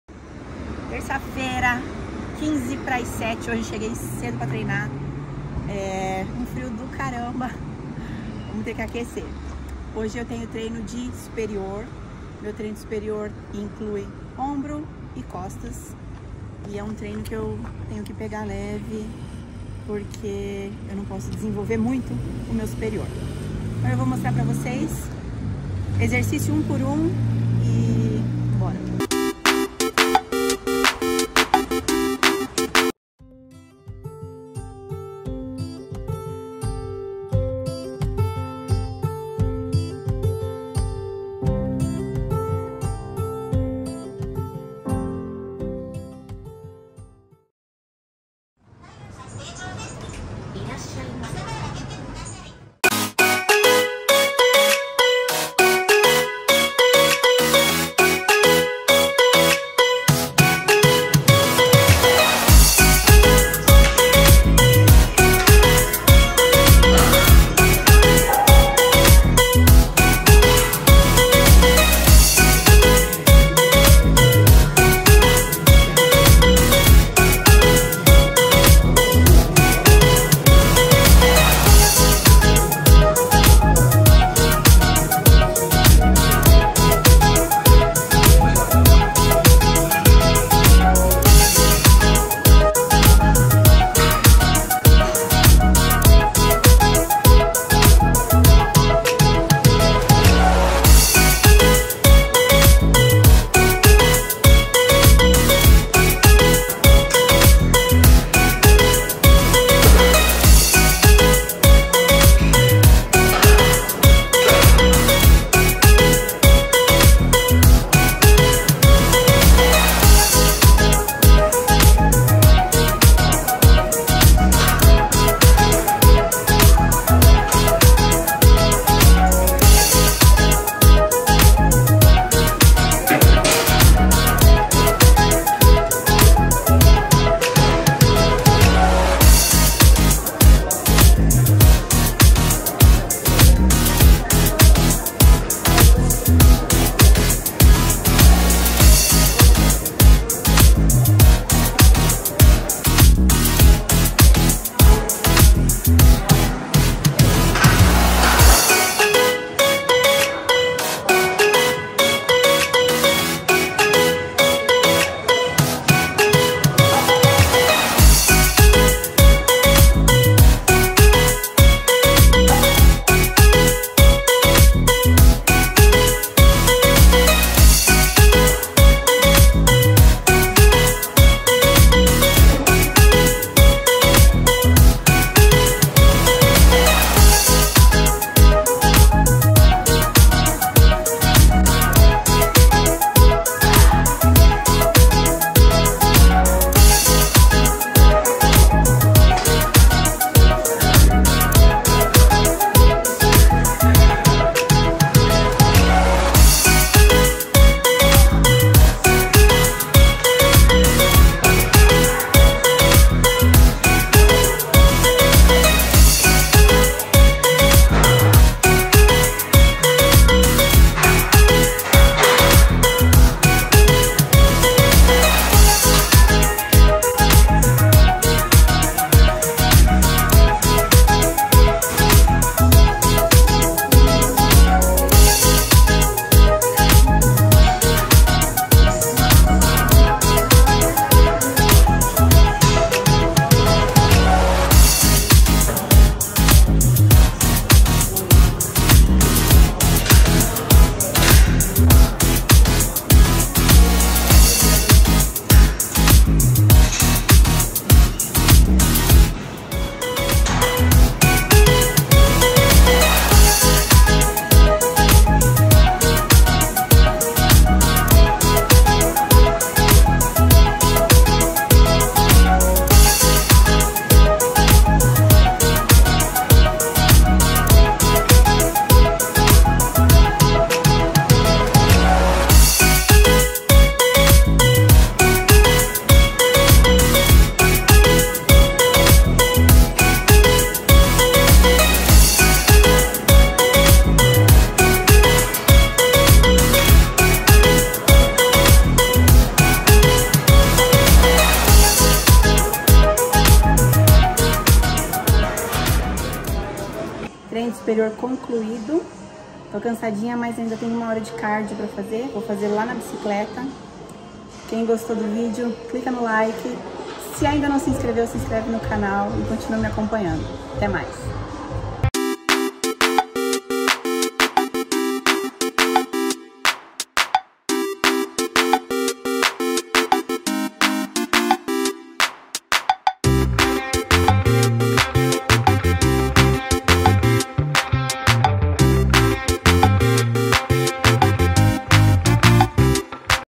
TREINO DE SUPERIORES DA SEMANA
Treino de superiores da terça-feira, dia frio mas nada impede a gente de treinar e destruir a musculatura!